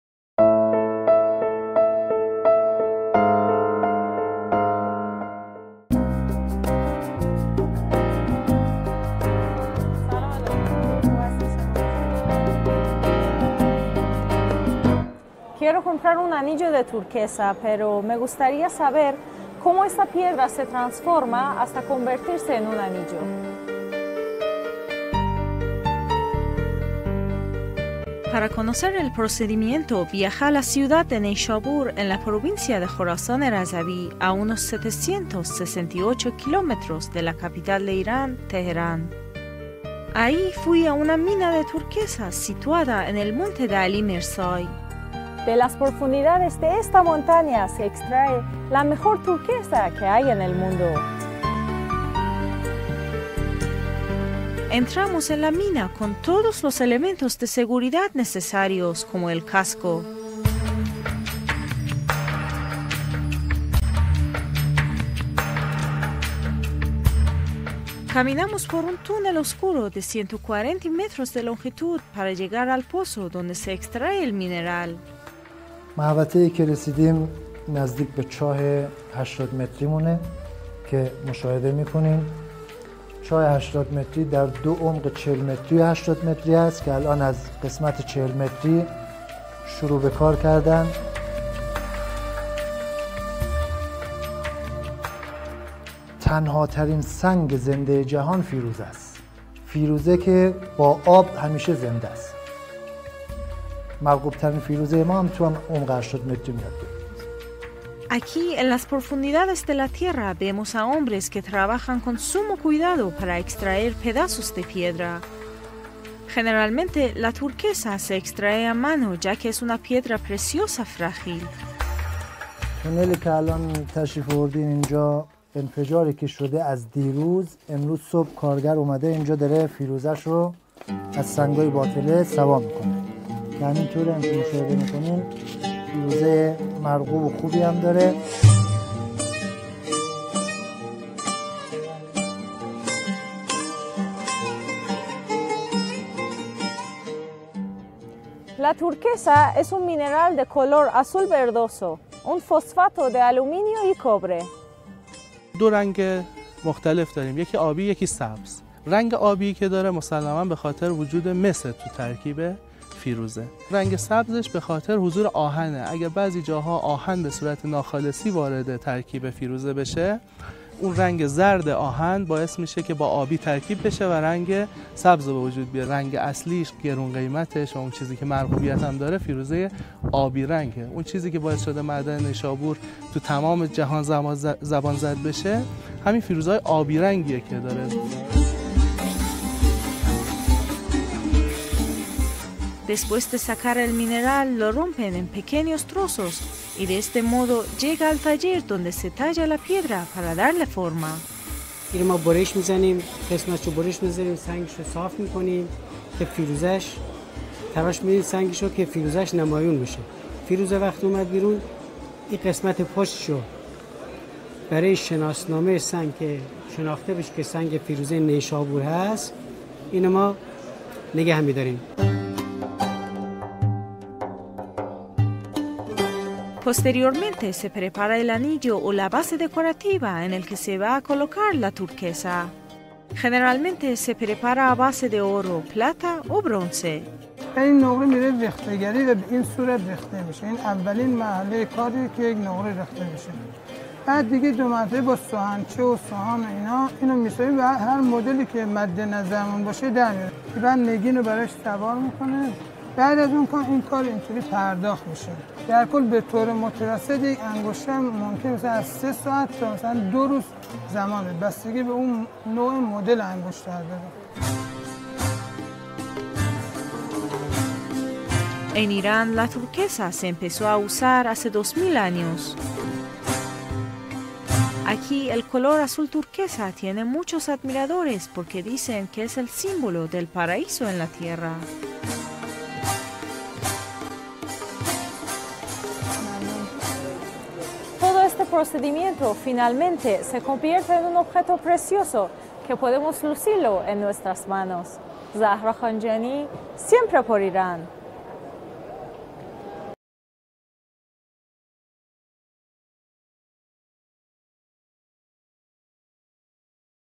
From Stone to Style Persian Turquoise Rings (Turquesa persa: de la tierra a la elegancia)
We will watch the depths of the Nishapur turquoise mines in Iran and learn about the origin of this rare blue gemstone in a fascinating documentary. Here, we will learn about the extraction of raw turquoise from the earth and follow its transformation from rough, unrefined nuggets into polished jewels fit for ornate handcrafted rings. Originally aired on Iranian television in Spanish, this behind-the-scenes look at the turquoise mining and jewellery-making process has been translated into English by Amitis Art. From the miners risking their lives to carefully retrieve the rough stones to the skilled artisans cutting and polishing them to reveal the dazzling finished gems, this short film shows the mesmerising progression of Persian turquoise from crude mine to exquisite adornment. Join this turquoise journey and gain an appreciation for the much-coveted Nishapur turquoise and the glimmering rings born out of age-old Persian craftsmanship in this one-of-a-kind documentary.
Observaremos las profundidades de las minas de turquesa de Nishapur en Irán y aprenderemos sobre el origen de esta rara piedra preciosa azul en un fascinante documental. Aquí, aprenderemos sobre la extracción de turquesa en bruto de la tierra y seguiremos su transformación desde pepitas en bruto y sin refinar hasta joyas pulidas aptas para anillos ornamentados hechos a mano. Transmitida originalmente en la televisión iraní en español, esta mirada detrás de escena del proceso de extracción y fabricación de joyas de turquesa ha sido traducida al inglés por Amitis Art. Desde los mineros que arriesgan sus vidas para recuperar cuidadosamente las piedras en bruto hasta los hábiles artesanos que las cortan y pulen para revelar las deslumbrantes gemas terminadas, este cortometraje muestra la fascinante progresión de la turquesa persa desde una mina tosca hasta un adorno exquisito. Únase a este viaje turquesa y aprecie la tan codiciada turquesa de Nishapur y los anillos relucientes nacidos de la antigua artesanía persa en este documental único.
00:28 Visiting the Nishapur mine
01:48 Collecting Turquoise
02:28 Turquoise Compositions & Colours
03:13 Cutting & Polishing Turquoise
03:33 How to Identify Nishapur Turquoise
03:47 Making Turquoise Rings
04:34 Symbolism of Turquiose